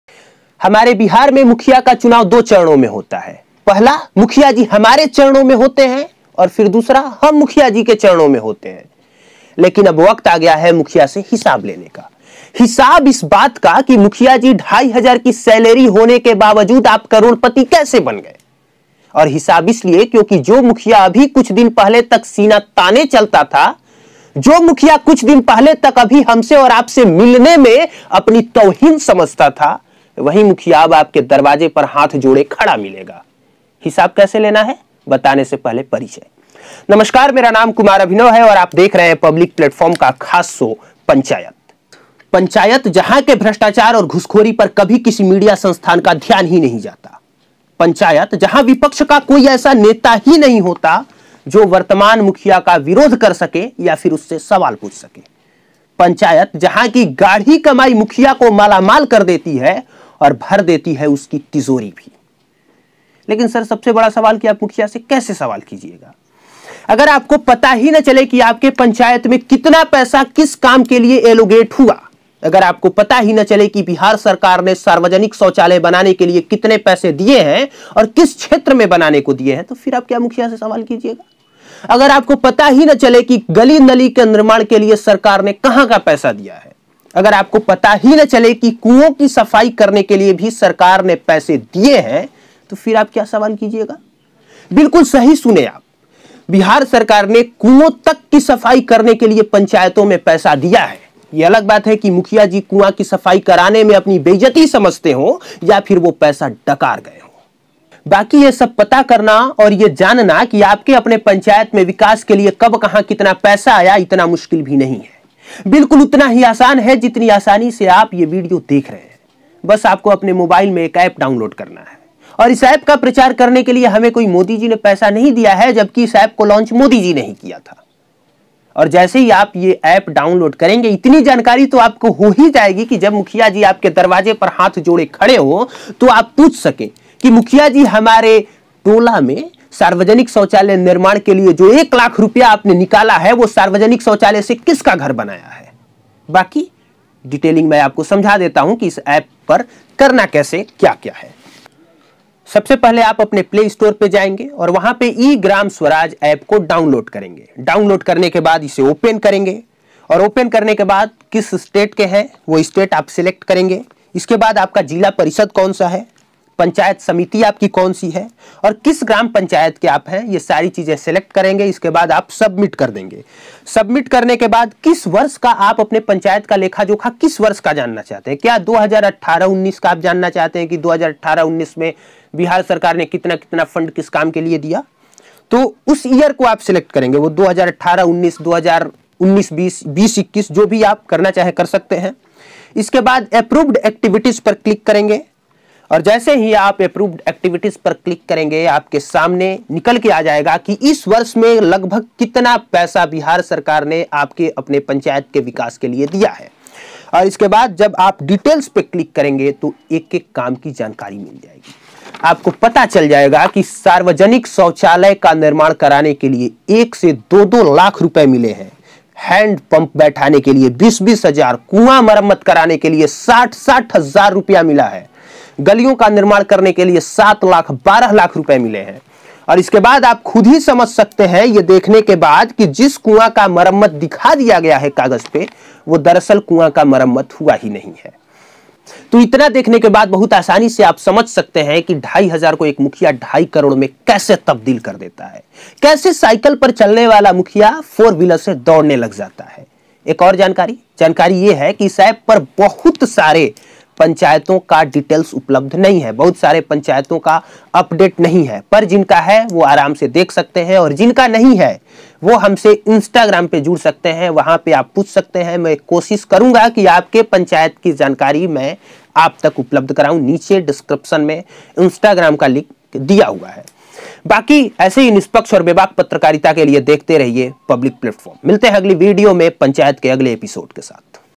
Panchayat Ep 2। आपके पंचायत में कब किस काम लिए कितना फंड आया ऐसे जानिए। Public Platform।
Hello
Namaskar Mai Kumar Abhinav

आज की Video में आप देख रहे हैं कि पंचायत का मुखिया/ प्रधान कैसे पैसों का गबन करता है।

जानिए इस वीडियो में की आपको कैसे पता चलेगा ... की आपके अपने पंचायत में कब किस काम के लिए कितना फंड आया.